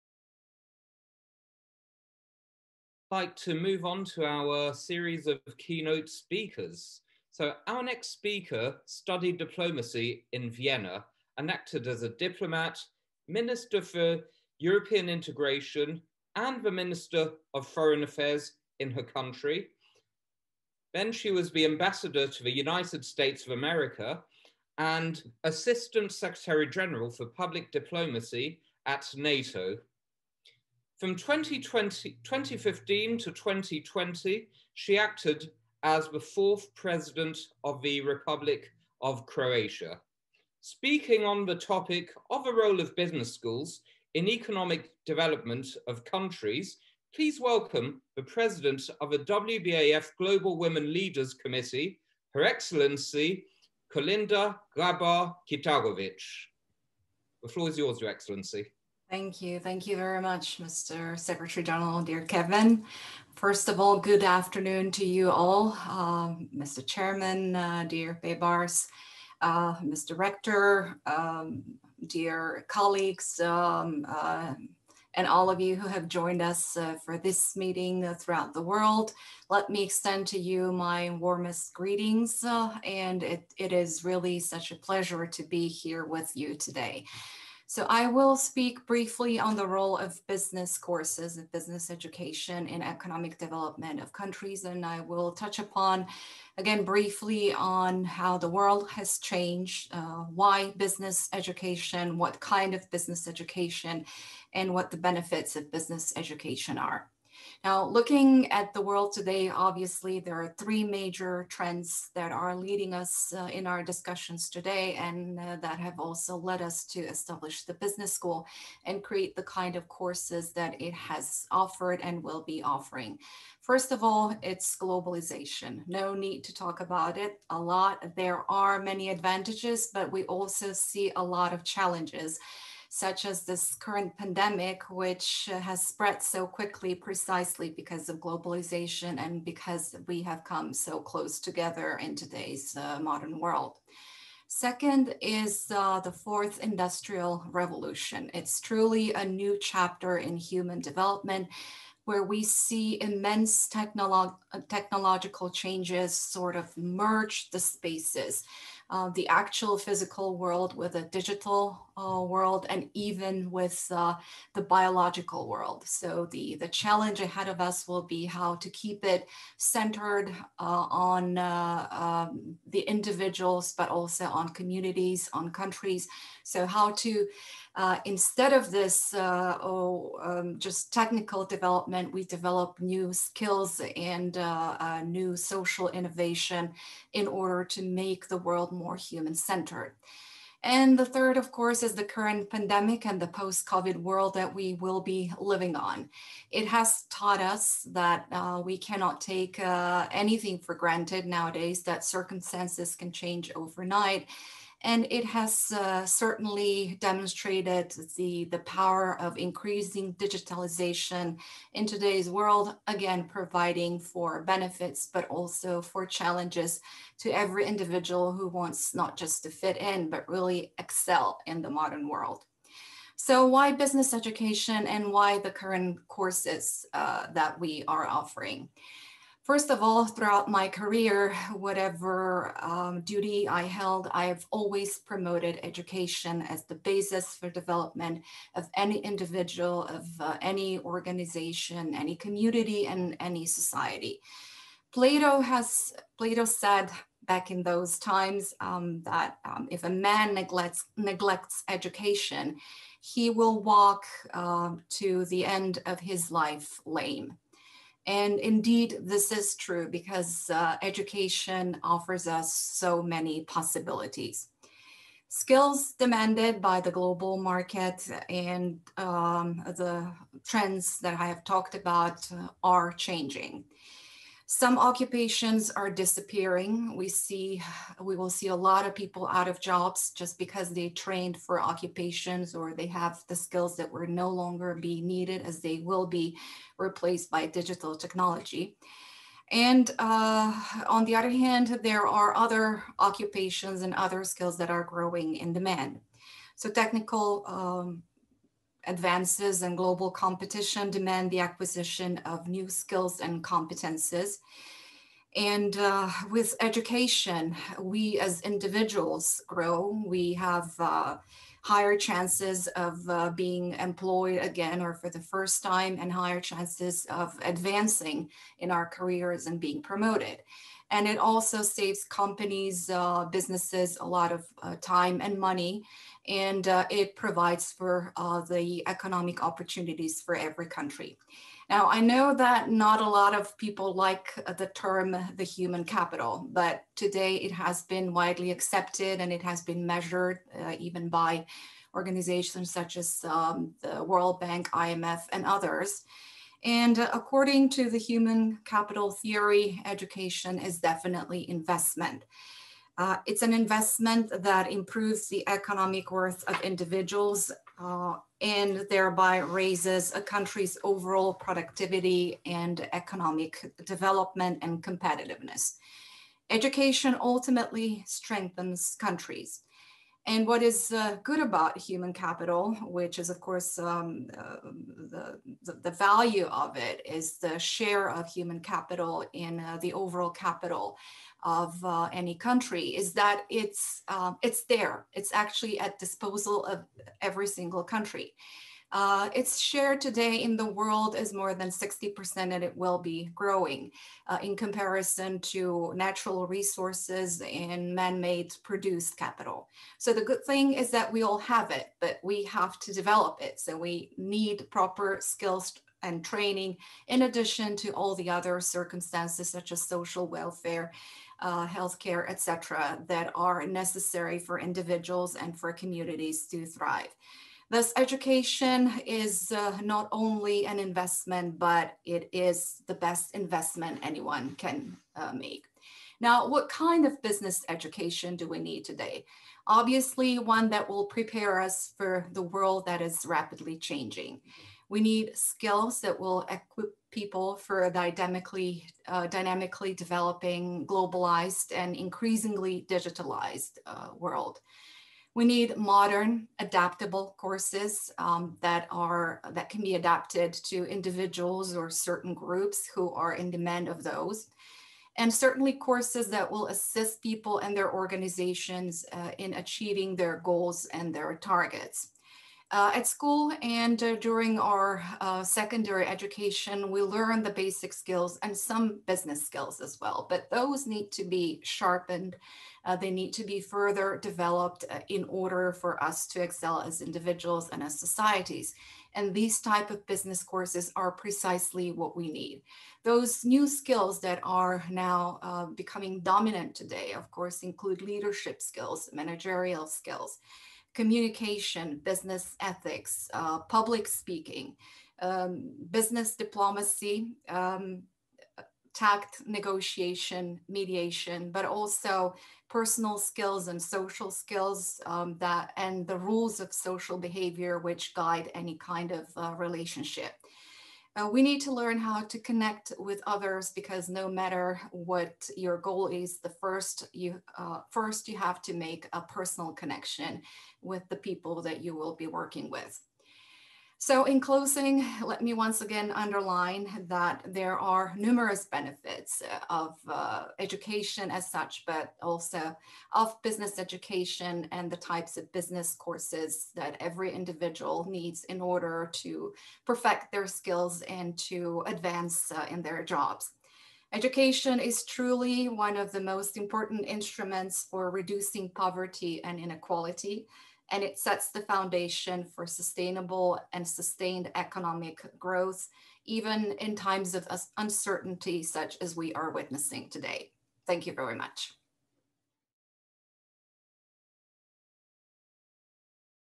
H. E. Kolinda Grabar Kitarović / Grand Openin Ceremony
2.30pm GMT
Keynote Speech: Role of business courses in economic development of countries
H.E. Kolinda Grabar-Kitarović, President of the Republic of Croatia (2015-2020)